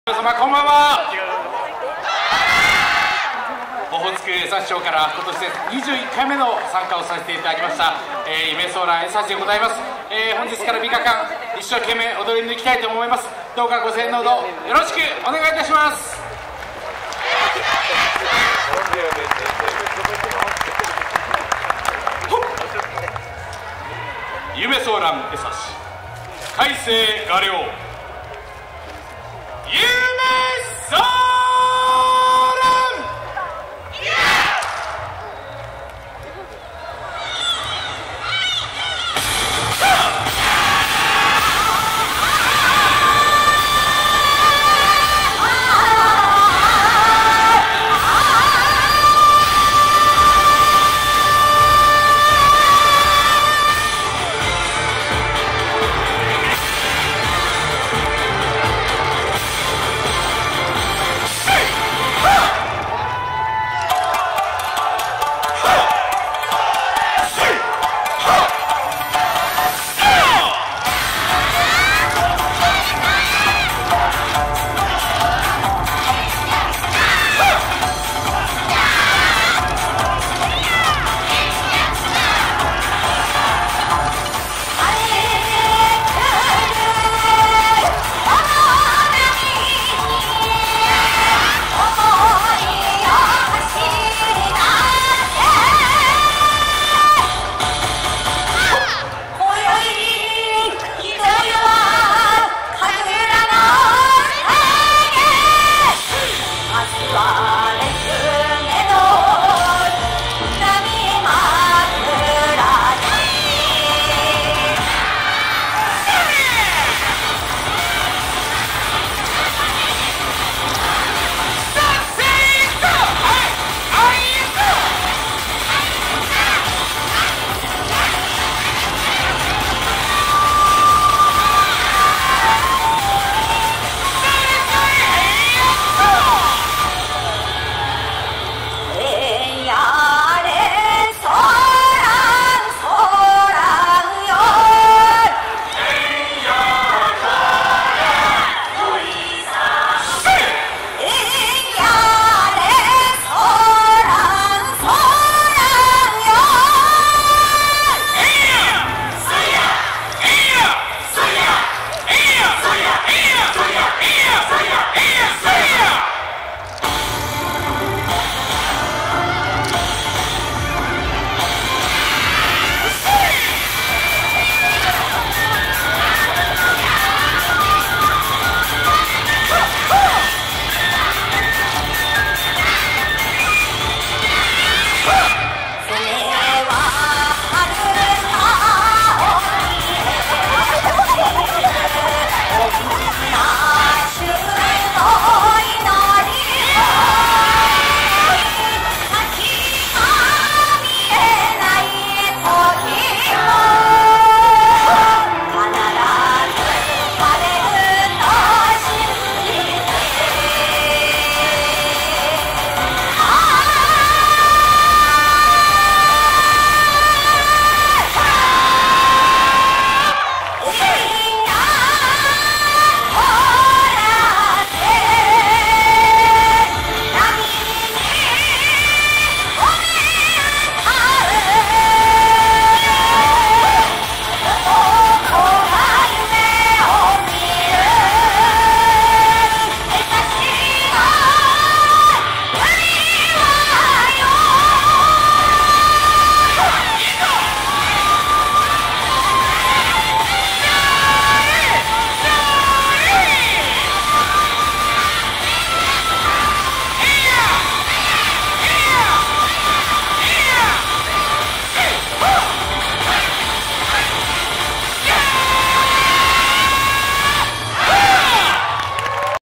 26th Yosakoi Soran Festival Sapporo 2017
another performance from the 26th Yosakoi Soran Festival in Sapporo. the video quality isn't all too good, but I wanted to share today's experience with my friends and family
the video quality looked somewhat better on my computer.